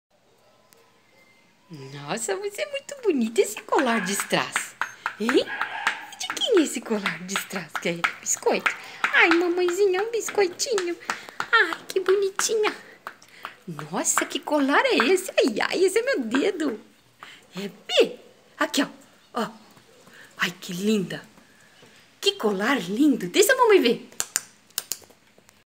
HOLLY
Estes 8 anjinhos lindos nasceram dia 03/09/18. São 3 machinhos e 5 fêmeas. Todos lindos, fortes e saudáveis. Não serão de porte pequeno caso seja esta sua busca, serão de porte médio. Eles tem patonas! Ou seja, serão bem vindos adotantes que tenham um pouco de espaço e algum tempo disponível. Pois são filhotes e demandam grande atenção neste começo de vida, não dá pra deixar muitas horas sozinhos. Contato para adoção